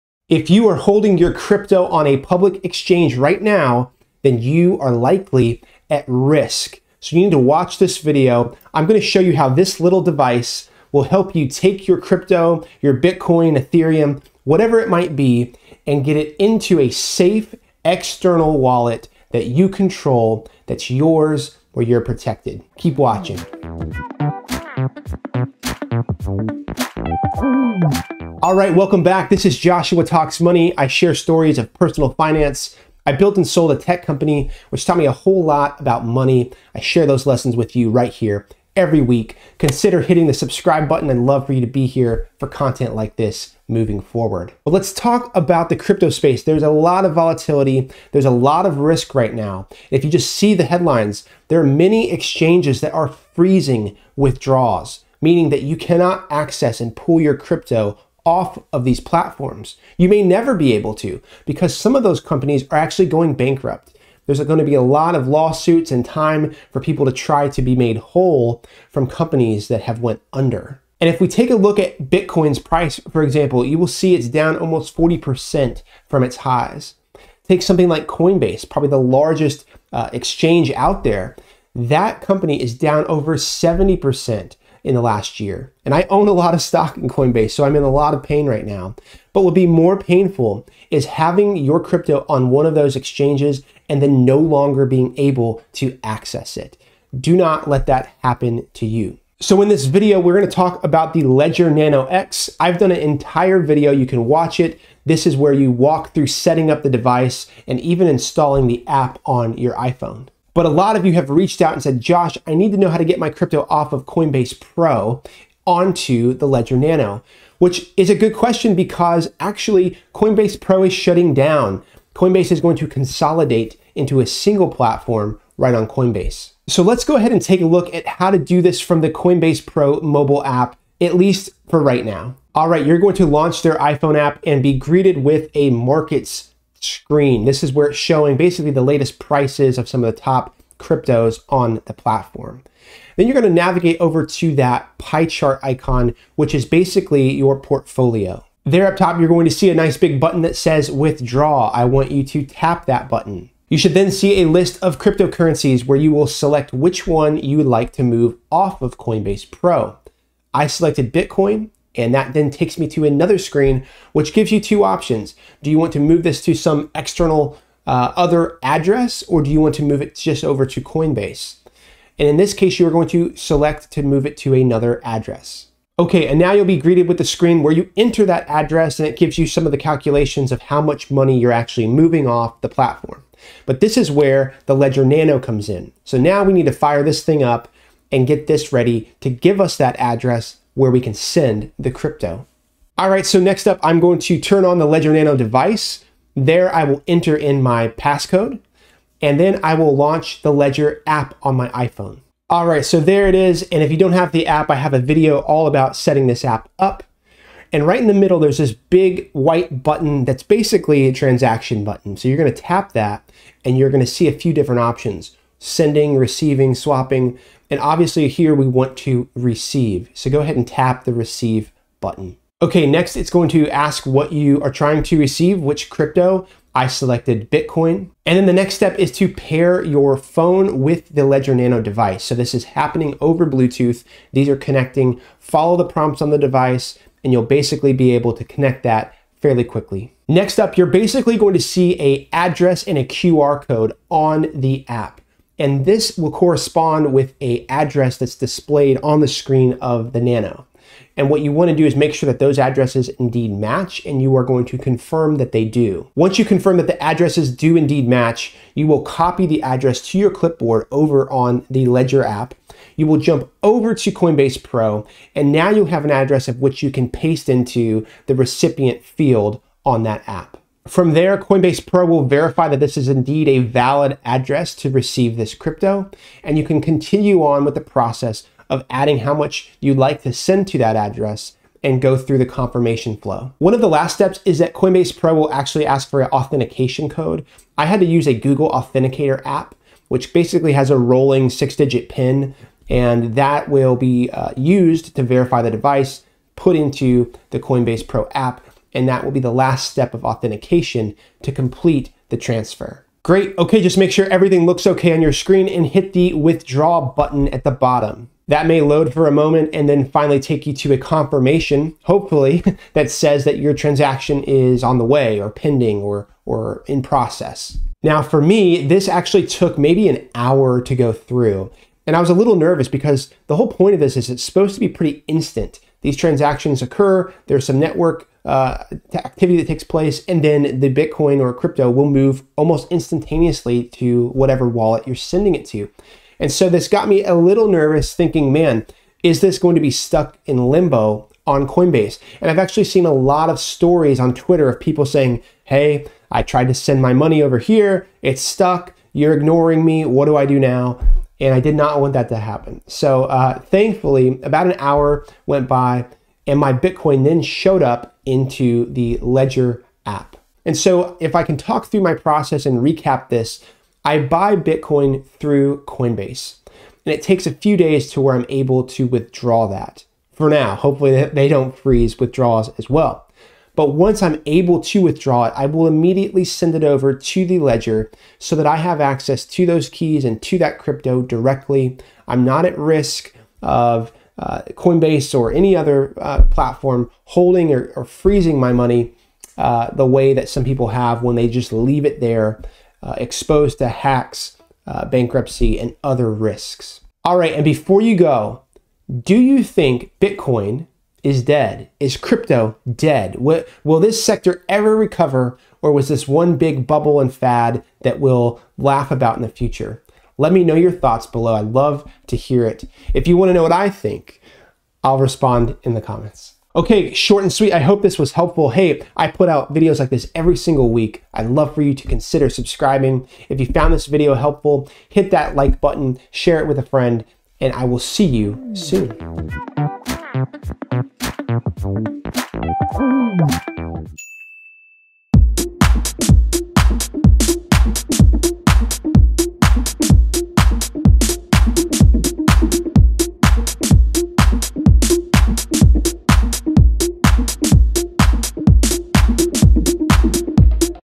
How to SAFELY store Crypto (DO THIS NOW)
🔐 Buy official Ledger devices 🔐

In this video, I talk about how to safely store your crypto like Bitcoin and Ethereum off of exchanges like Coinbase Pro by using the Ledger Nano X. The Ledger Nano X is a hardware crypto wallet that allows you to easily store your crypto and gives you full access to your keys.

There is a lot of uncertainty in the crypto markets with Bitcoin down nearly 40%, Coinbase stock ($COIN) is down over 80% from the highs, and new stories about exchanges like Celsius, Voyager and others pausing withdrawals from their platforms. Imagine having tens of thousands of dollars in crypto on a public exchange and being told you can no longer access and withdraw that money!

There's also news that Coinbase Pro will be shutting down soon. You need to protect yourself and get control of your crypto assets with a hardware wallet. I'll walk you through the process, screen by screen, of how to safely move Bitcoin from Coinbase Pro to your Ledger Nano X.

0:00 - Intro
0:44 - The Risks of Exchanges
1:39 - How to Stay Safe
2:13 - Withdrawal from Coinbase Pro
3:03 - Send to Ledger Nano X
4:16 - Confirming Your Transfer
5:18 - Waiting for Confirmation
6:00 - Recap of the Process